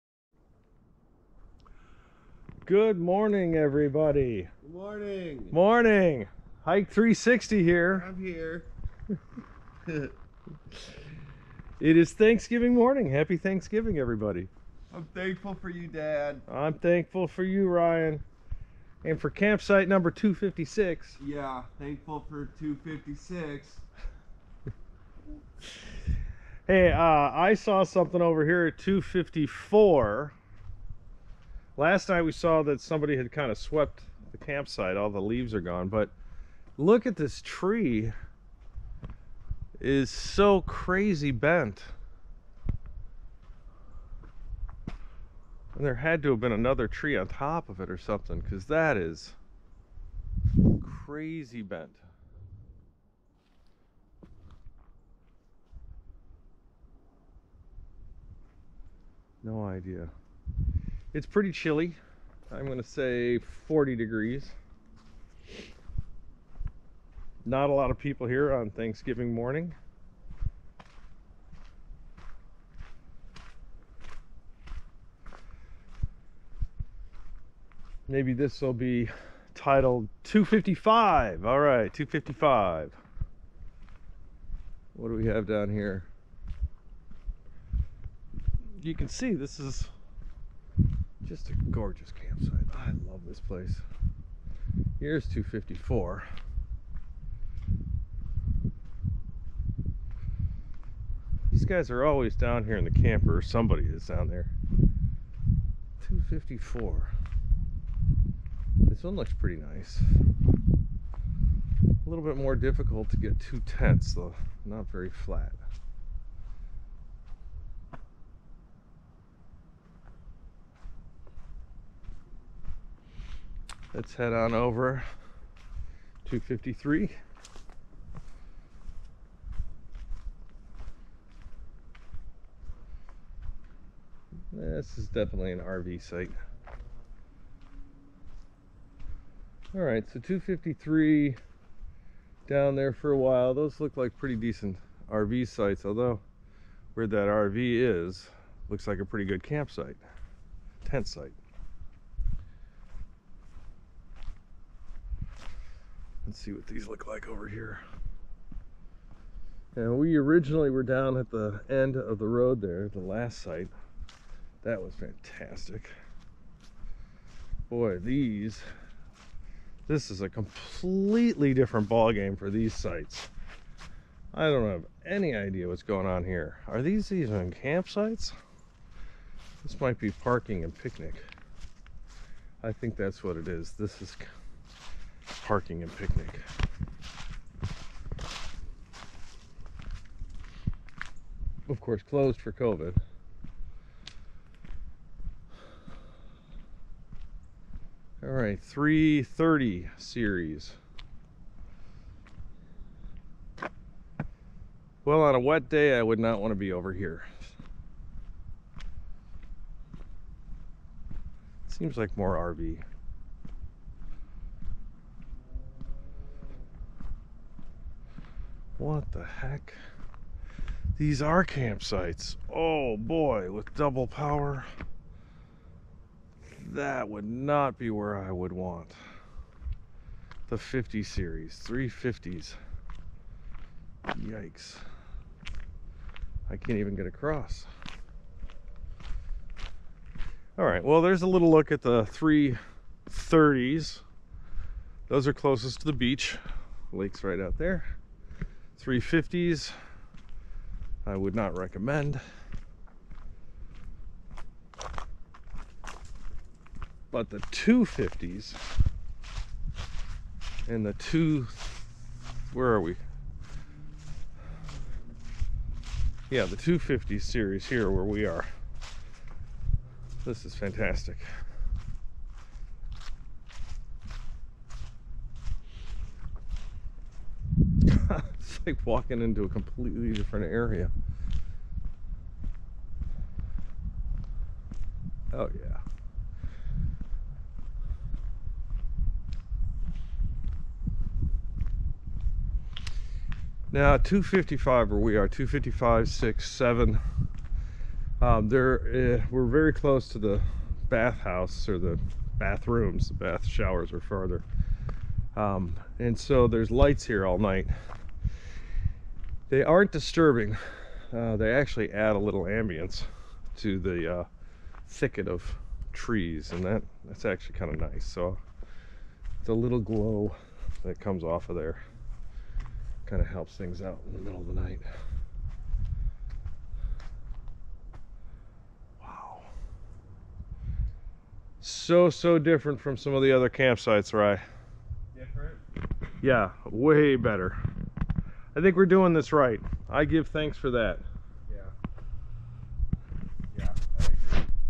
IL Beach State Park - Winter Camping Thanksgiving Morning (Hike 360° VR Video)
Experience a Thanksgiving morning hike with beautiful scenery and unique campsites at Campsite Numbers 254, 253, 350s, and more. Join us as we explore the diverse locations and charming features of each site, providing insights and recommendations for your next outdoor adventure. Watch as we uncover hidden gems and share our gratitude for nature's beauty. Don't miss this captivating journey into the wilderness with Morning Hike 360!

We woke up on Thanksgiving morning, in the woods, at a State Park. It doesn't get much better than that. Also, check out that fog! We are exploring campsites 254, 255, 256, 257 in this video as well as campsites 330-350.

Timestamps:
1. Thanksgiving Morning Hike and Campsite Tour - 00:00:02
2. Mysterious Tree at Campsite 254 - 00:26
3. Exploring Campsite 255 - 01:21
4. RV Sites at Campsite 253 - 02:03
5. Different Types of Campsites - 02:36
6. Parking and Picnic Areas - 03:02
7. Overview of Campsite Series 330 - 03:18
8. Campsite Series 350 Review - 03:54
9. Fantastic Campsite Series 250 - 04:20
10. Campsite 255 Features and Amenities - 04:58
11. Comparing Different Campsite Experiences - 05:32

youtu.be/RbyWVnqB6ak?si=uSMQySp6RE1W7ck2 Des Plaines River Trail, IL
Winter Hike at Moraine Hills State Park, IL